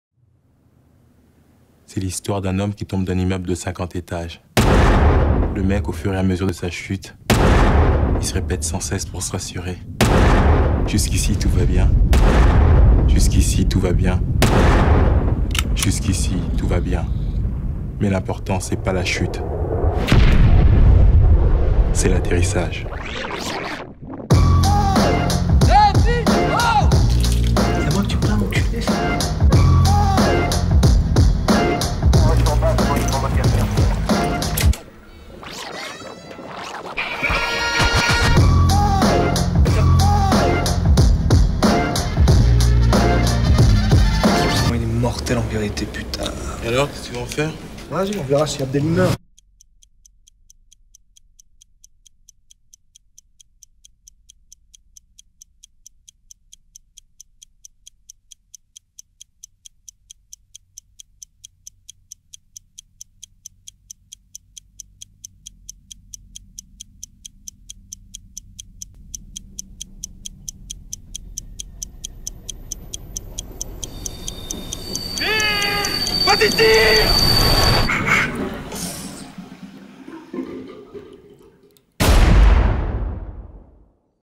La Haine (Sweded)
Students recreate a sweded trailer for 90's French Classic "La Haine"